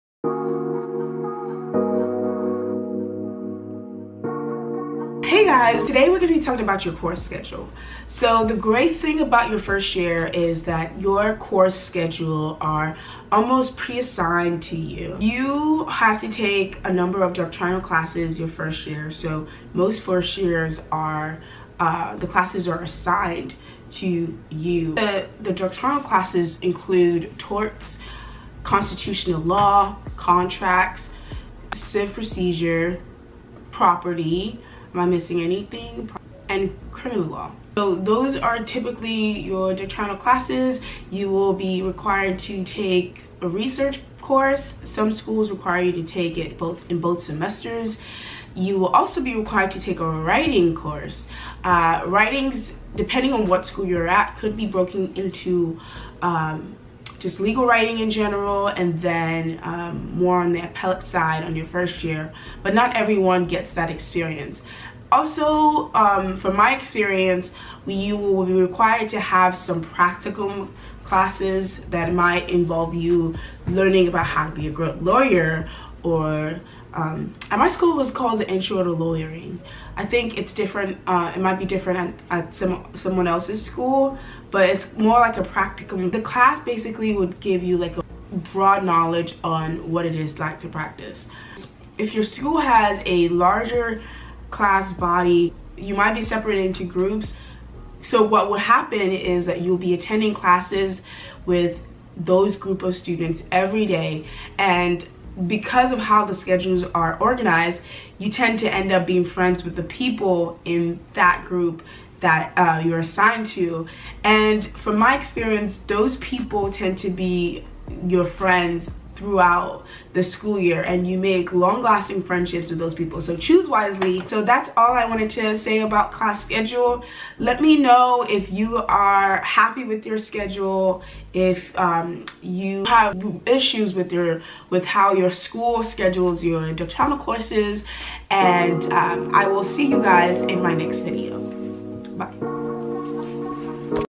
1L Class Schedule
Hey guys, check out what I have to say on the first-year law class schedule. Join the conversation.